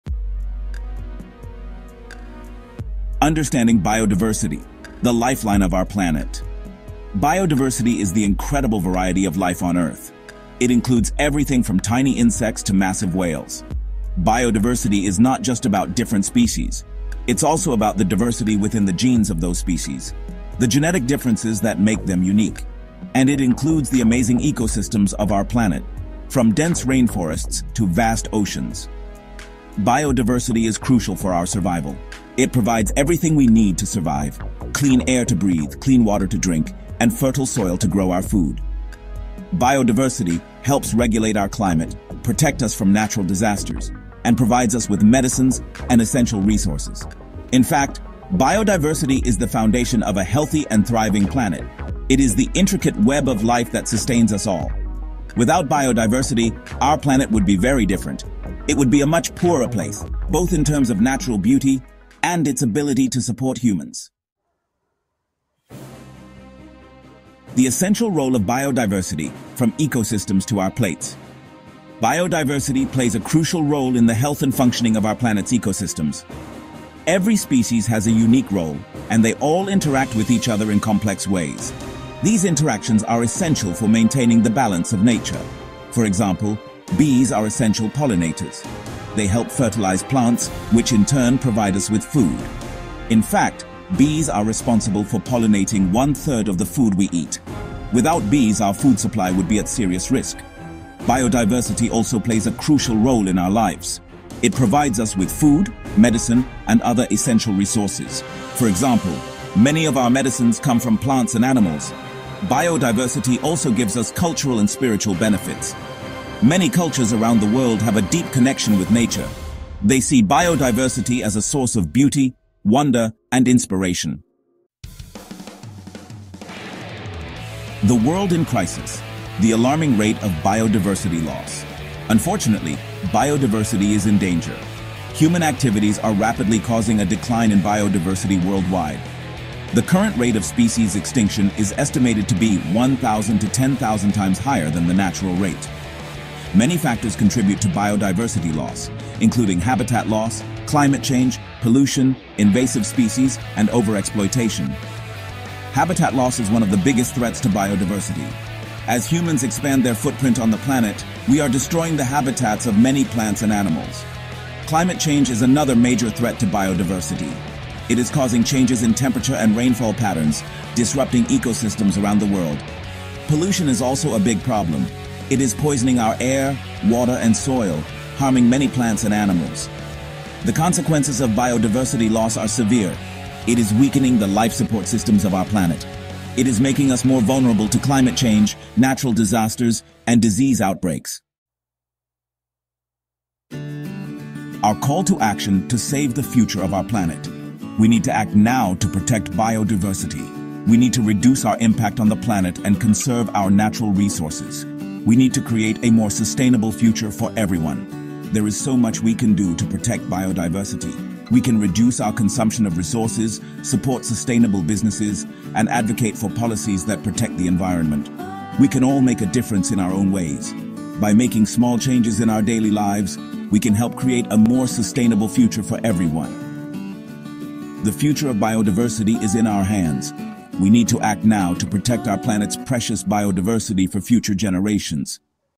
The Biodiversity Crisis: Causes and Effects (English)
Did you know that the biodiversity crisis is a serious threat to our environment and lives? In this 3-minute video, we will discuss the causes and effects of biodiversity loss. We will learn how the destruction of natural habitats, climate change, and human activities are endangering the existence of various species.
What steps should we take to protect our nature? Learn simple measures and solutions in the video so that we can save our environment.